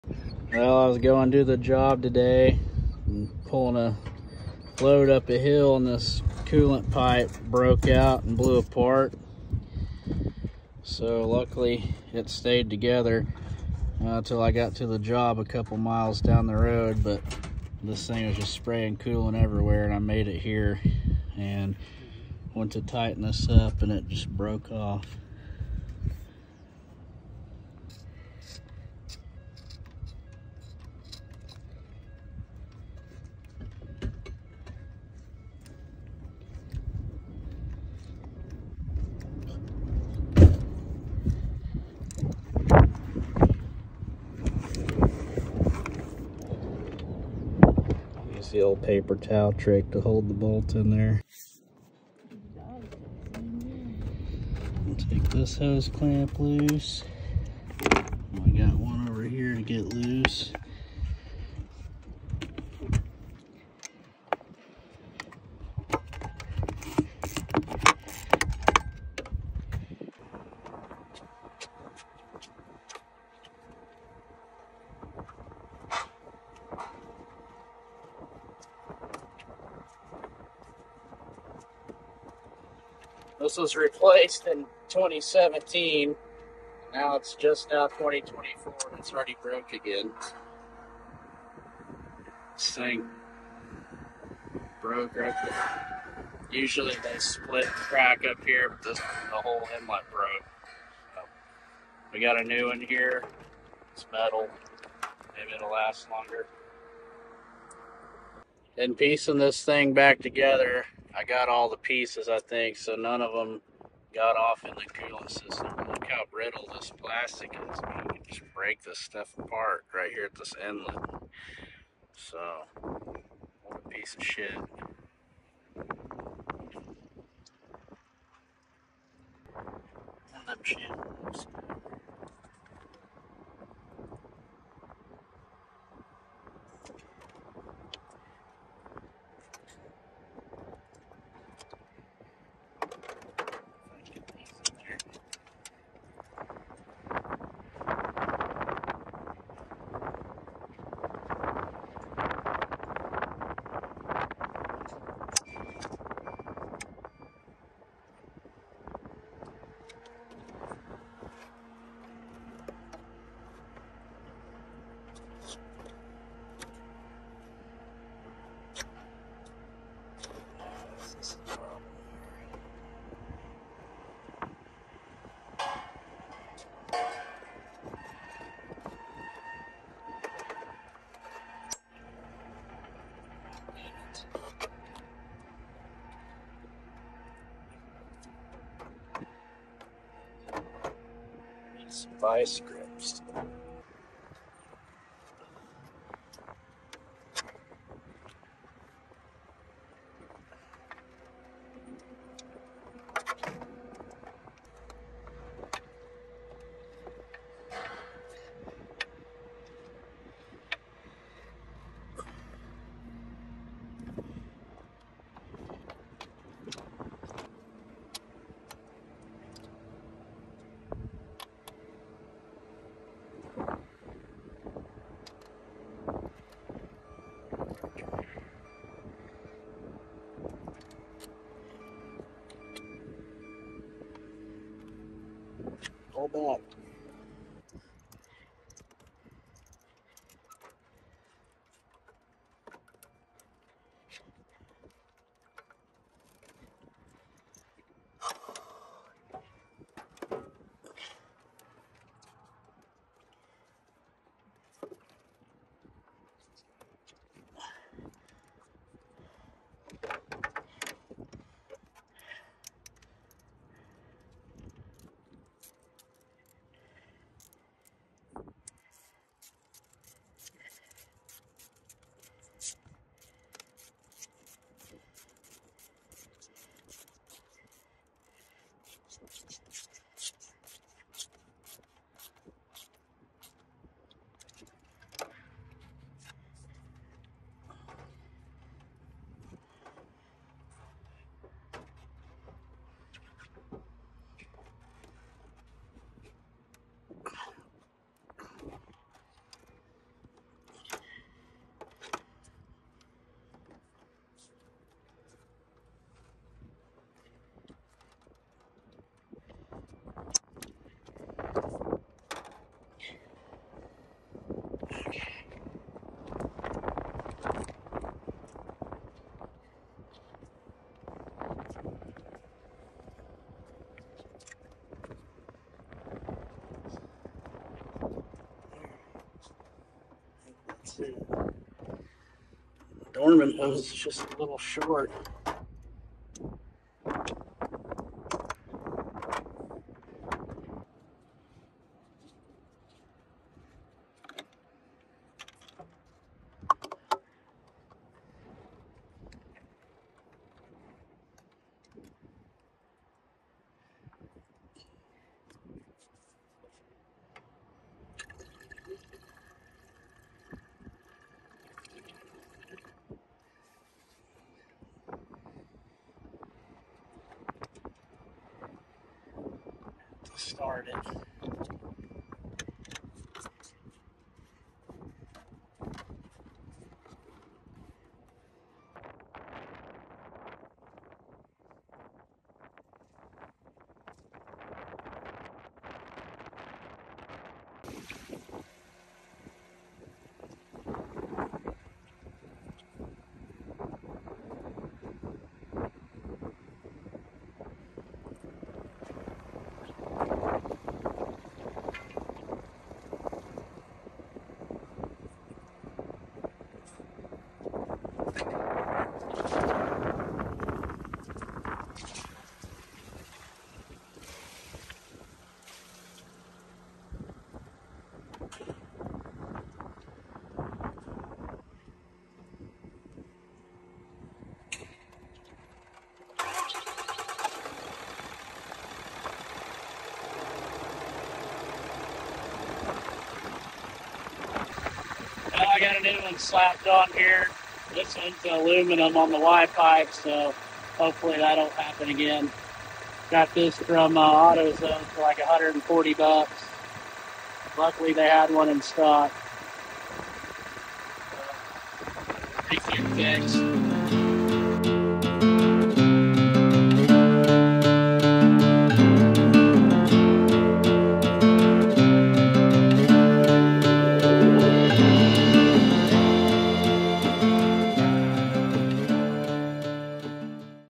Broke Down On The Job - Ram Cummins Coolant Y Pipe Failure & Fix
I was pulling the skid steer to a job noticed that my coolant temp started rising and I started smelling coolant. So I pulled over noticed a coolant hose spraying coolant out from the left side up above the alternator I was about 2 miles away from the job luckily so I just eased it on over there. The hose hadn't came completely off so I thought I'd get lucky and tighten up the hose clamp and when I went to grab the hose to tighten it it just crumbled and you could fill the hose just kind of poped off so then I pulled it off and seen that that plastic y pipe was all cracked. I called around a couple ram dealers and nobody had one in stock I found one online at AutoZone and my local AutoZone actually had this part in stock they got is like 140 bucks but it was a doorman brand and it came with an aluminum white pipe so I swapped this out because this is the second time that this is happened with the plastic one so hopefully this aluminum piece takes care of the problem pretty easy job to do just need a little socket set and some vice grips.